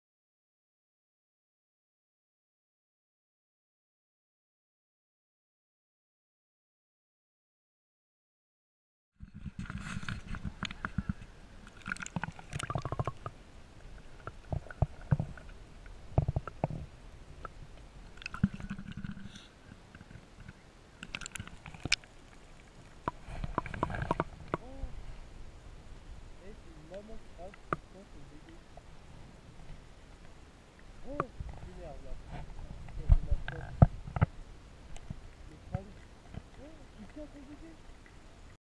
Underwater test (GOPRO HERO entry level)
Underwater test with the Gopro hero entry level at the sea ^^ :D
720p60